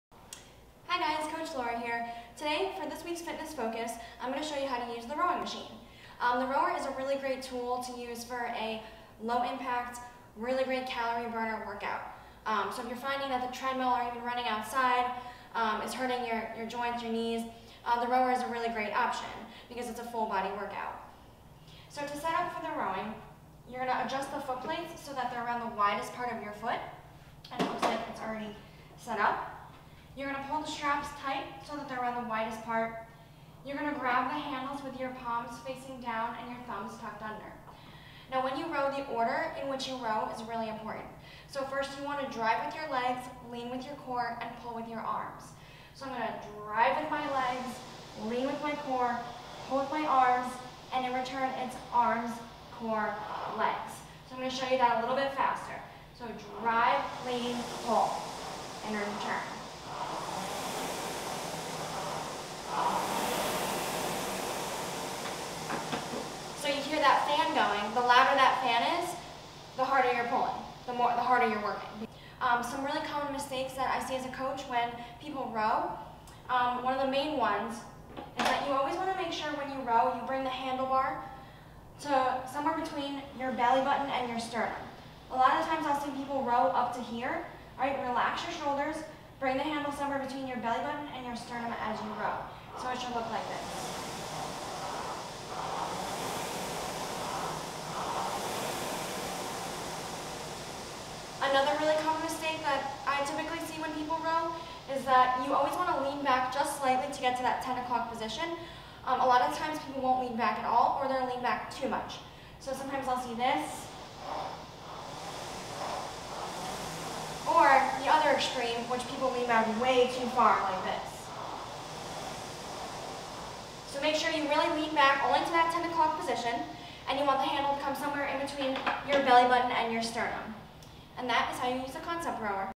How to Properly use the Concept Rower Machine (Common Mistakes when using the rowing machine)
Rowing is a great alternative to running because it is low impact and burns a lot of calories. Rowing uses over 80% of the muscles in your body making it a full body workout. Mastering form on the rowing machine is a challenge for many people because the order, power, and timing is overlooked. When rowing, first you need to drive with the legs, lean back using the core, and lastly, pull with the arms. In return, the order is arms, core, legs. The majority of the power will come from the legs and the remaining and subsequent power will come from the core then arms. At Next Fit Clubs, we use the Concept rower machine. As you start to row the screen will automatically turn on. The most important numbers that should be looked at are the time, found at the top left, the meters which is typically shown in the middle of the screen depending on the display, and the “/500m” split time, which basically shows how hard you are working.

Common Mistakes:
Leaning back too far
-Correction: make sure your feet have full contact with the foot plates
Bringing the handle to the chin
-Correction: Relax the shoulders down and away from the ears, bringing the handle between the belly button and sternum
Rowing too fast
-Correction: Tempo is just as  important when you row as when you lift. In rowing, it’s one count as you push away, and two counts on the return.